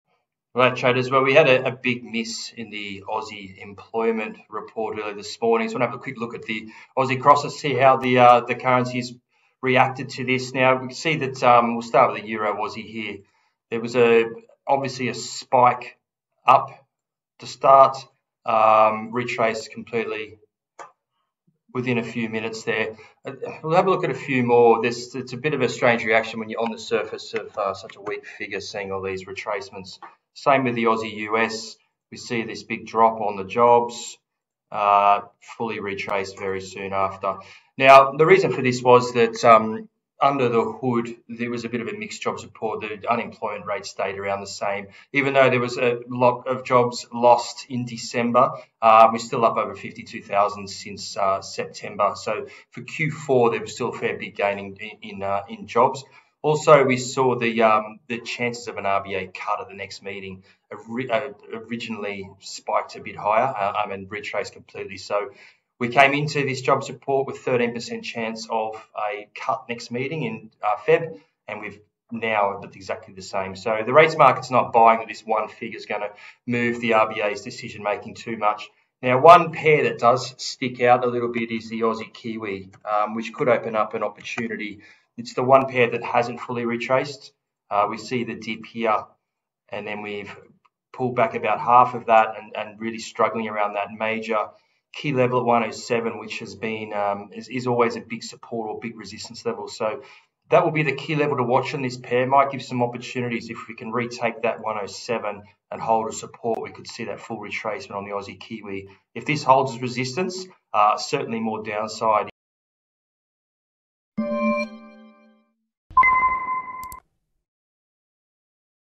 AUD reaction to Jobs report
A big miss in todays Aussie employment report for December. AUD drops post announcement but retraces as rates market not convinced this one report will move the needle on the RBA's next decision. AUDNZD the odd one out, opening up a trade opportunity.